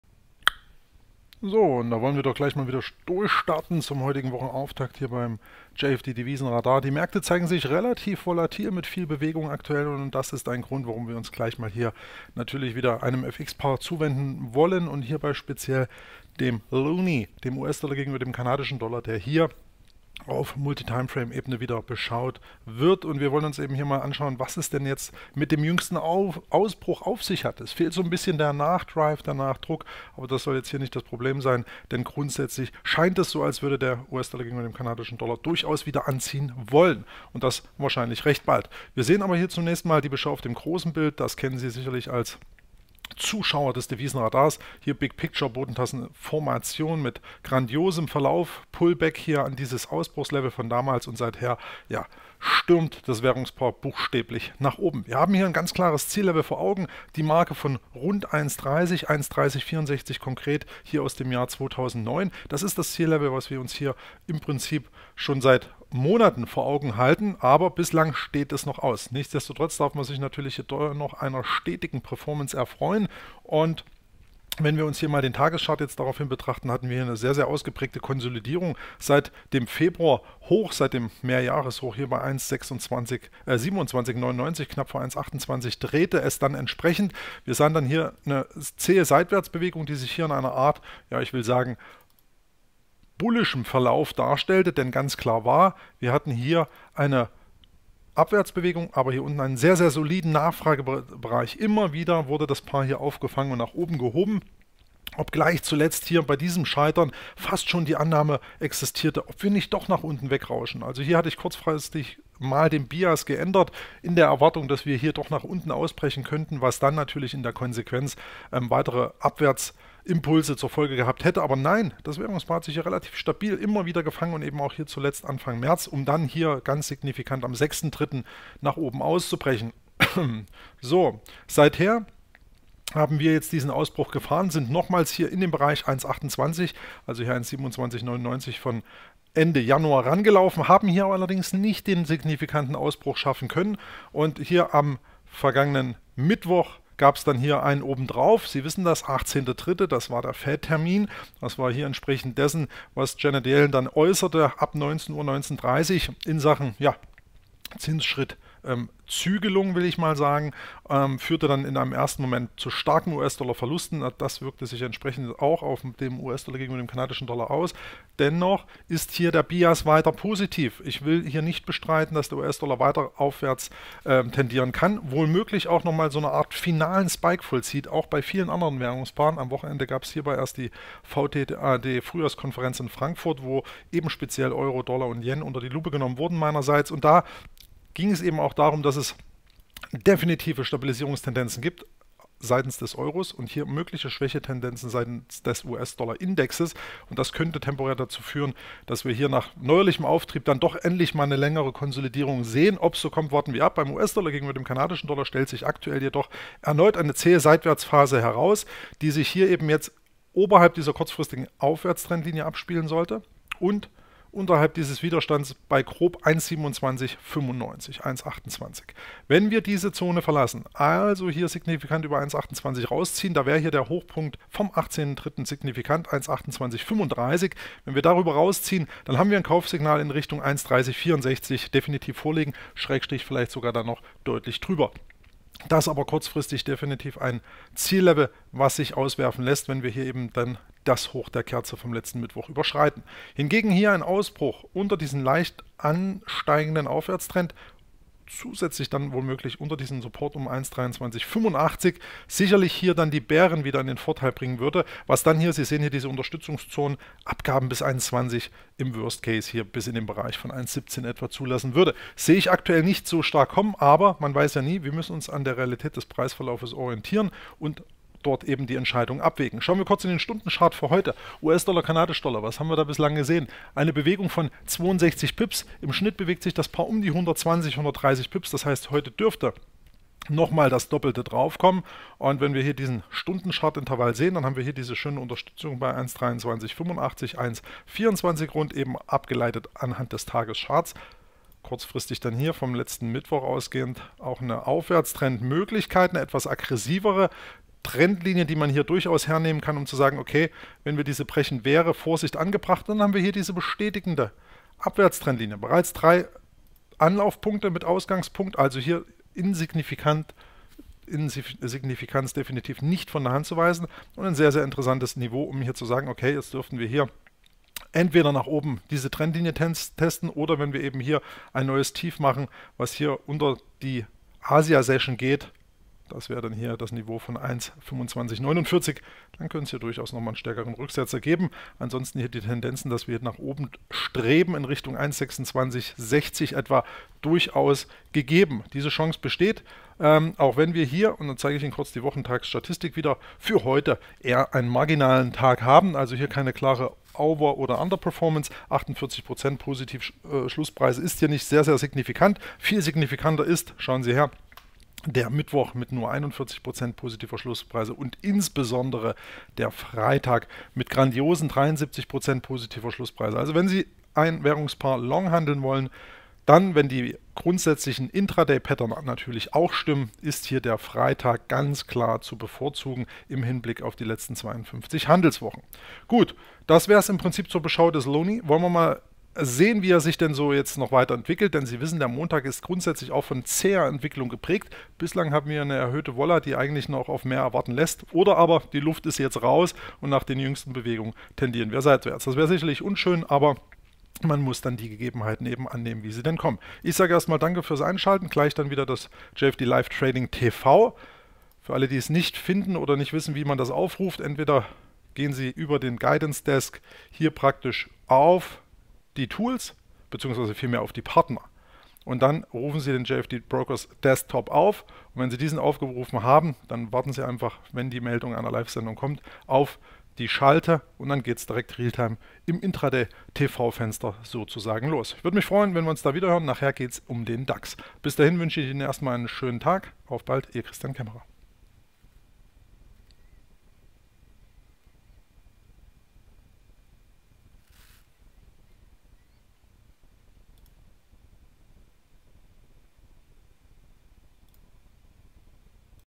JFD Devisenradar: USD/CAD (Loonie) mit der Ruhe vor dem Sturm
Der Loonie Anfang März nach oben hin ausbrechen, doch so recht wurden weitere Aufwärtssziele bislang nicht